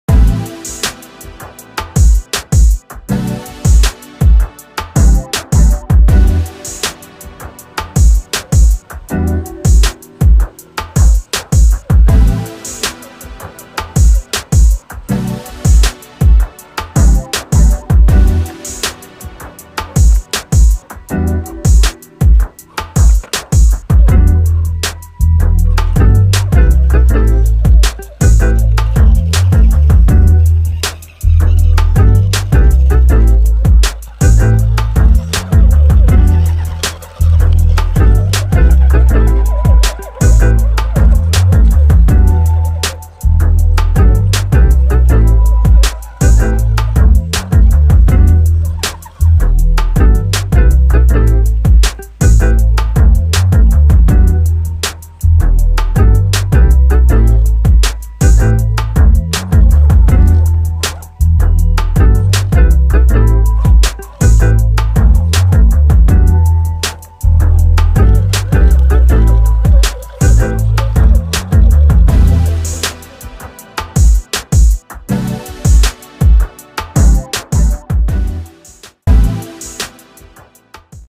RC Jeep Rubicon Hard Body
Green Jeep Rubicon hard body on 55t 2s Lipo Bat.
Chasis mod due to a shorter wheelbase.
First run with this setup and i will go through all the changes is have done to fit this body especially coming from the f100 ford truck which is 2 inches longer.